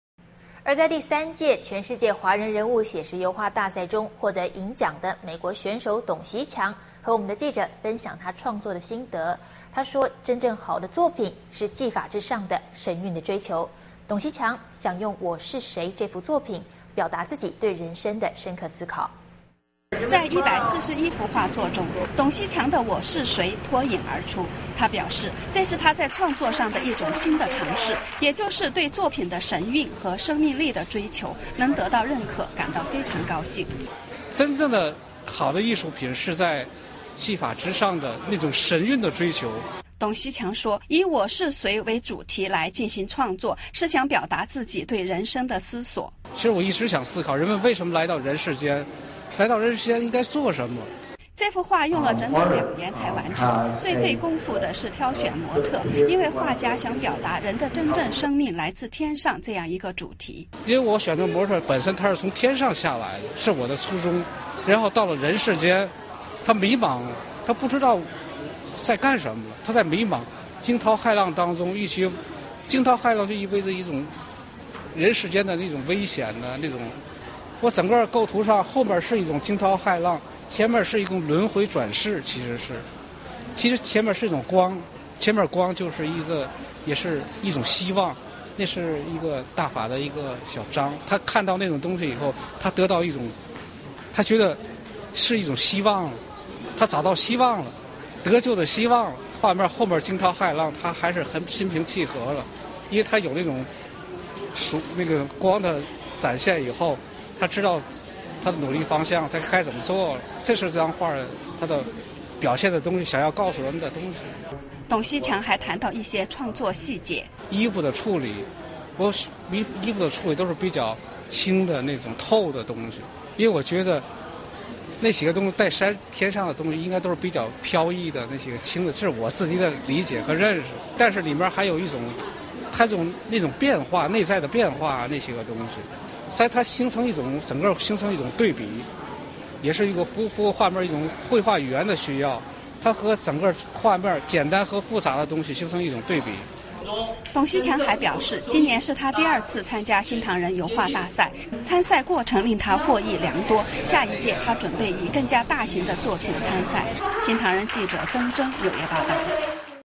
油畫大賽銀獎得主 談作品《我是誰》
【新唐人亞太台2011年11月30日訊】而在第三屆「全世界華人人物寫實油畫大賽」中獲得銀獎的美國選手董錫強，與我們的記者分享創作的心得，他說，真正好的作品是技法之上的神韻的追求。董錫強，想用《我是誰》這幅作品，表達自己對人生的深刻思考。

在141幅畫作中，董錫強的《我是誰》脫穎而出。他表示，這是他在創作上的一種新的嘗試，也就是對作品的神韻和生命力的追求，能得到認可，感到非常高興。

銀獎得主 董錫強：「真正的好的藝術品是在技法之上的神韻的追求。」

董錫強說，以《我是誰》為主題來進行創作，是想表達自己對人生的思索。

銀獎得主 董錫強：「我一直想思考，人們為什麼來到人世間，來到人世間應該做什麼。」

這幅畫用了整整兩年才完成，最費功夫的是挑選模特，因為畫家想表達「人的真正生命來自天上」這樣一個主題。

銀獎得主 董錫強：「我選擇模特，本身她是從天上下來的，是我的初衷，然後到了人世間，她迷茫，她不知道在幹什麼，她在迷茫，驚濤駭浪之中，驚濤駭浪就意味著人世間的危險。」

銀獎得主 董錫強：「整個構圖上，後面是一種驚濤駭浪，前面是一本《輪迴轉世》，前面是一種光，也是一種希望那是大法的一個小章，她看到那種東西以後，她覺得是一種希望，她找到希望了，得救的希望了。畫面後面驚濤駭浪，她還是很心平氣和了，因為她有那種光的展現以後，她知道努力方向，該怎麼做了。這是這幅畫表現的東西，想要告訴人們的東西。」

董錫強還談到一些創作細節。

銀獎得主 董錫強：「衣服的處理，都是比較輕的，那種透的東西，因為我覺得天上的東西都是比較透的，飄逸的，這是我自己的理解和認識。但是裡面還有一些內在的變化，它整個形成一種對比，也是符合一種繪畫語言的需要，簡單和複雜的東西形成一種對比。」

董錫強還表示，今年是他第二次參加新唐人油畫大賽，參賽過程令他獲益良多，下一屆他準備以更加大型的作品參賽。

新唐人記者 曾錚 紐約報導